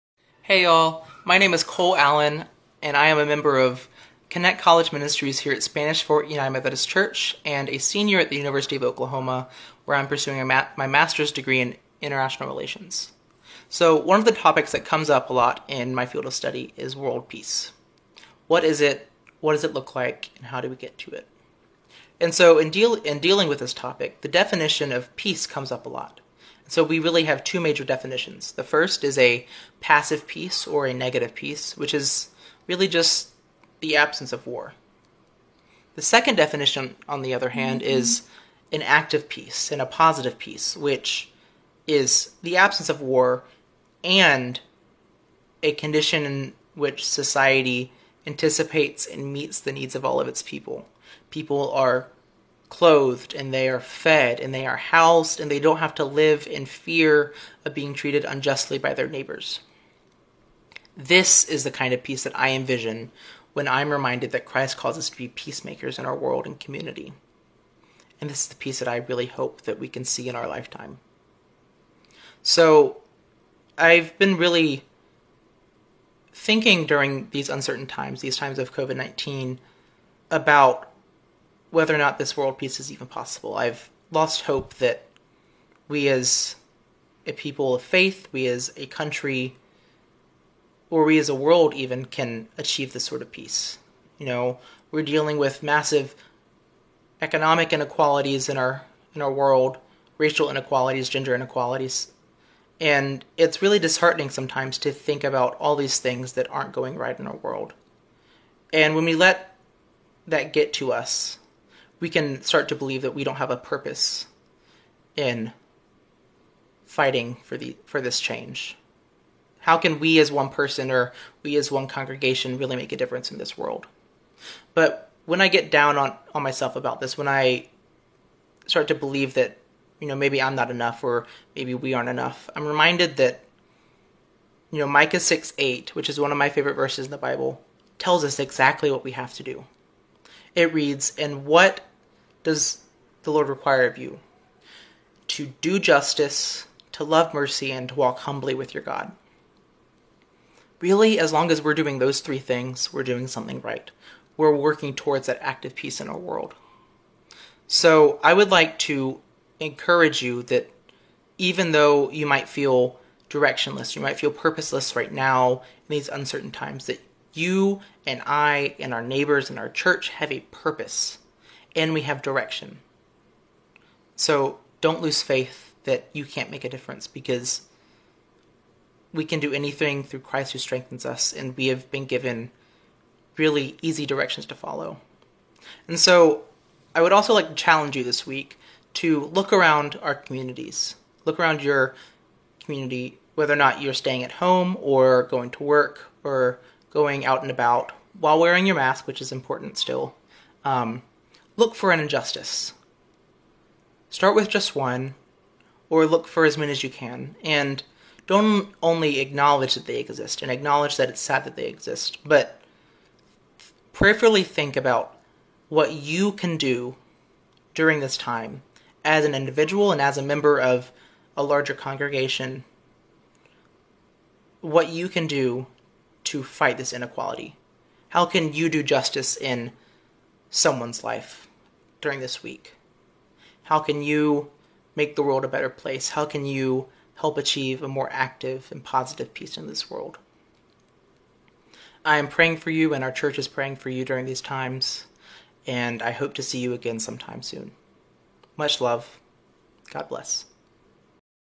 Teen Tuesday | Midday Message | 2020-08-11
Cole Allen delivers our Teen Tuesday today.